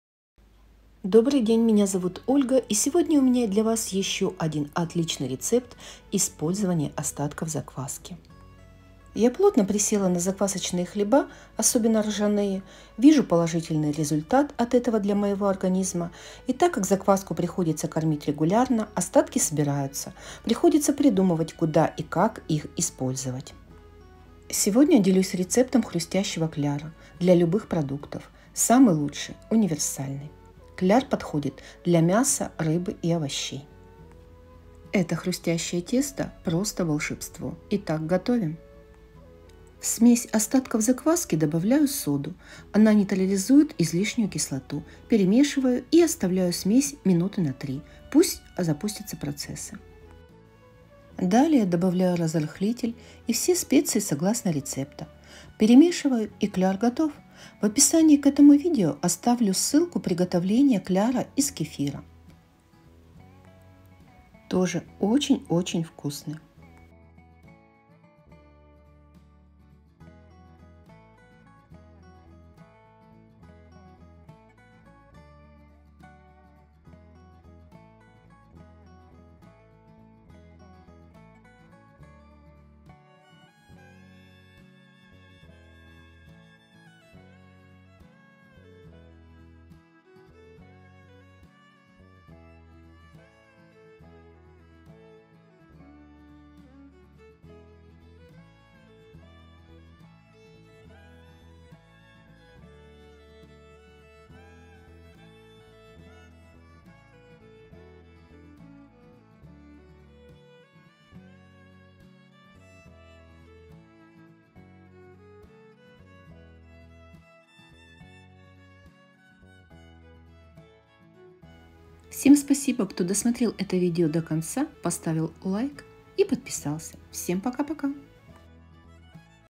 ХРУСТЯЩИЙ КЛЯР ДЛЯ ОТБИВНЫХ НА РЖАНОЙ И ПШЕНИЧНОЙ ЗАКВАСКЕ
Вкусный кляр, как в KFC

Хрустящий кляр
500гр закваски (остатки)
1 чайная ложка соды
Перемешать и оставить на 3 минуты,
далее
1 яйцо
1 столовая ложка тертого свежего лука
1 чайная ложка горчицы
1 чайная ложка пасты острой паприки
0,5 чайной ложки молотого имбиря
0,5 чайной ложки сухого чеснока
0,5 чайной ложки молотого черного перца
1чайная ложка без горки разрыхлителя для теста
Соль по вкусу.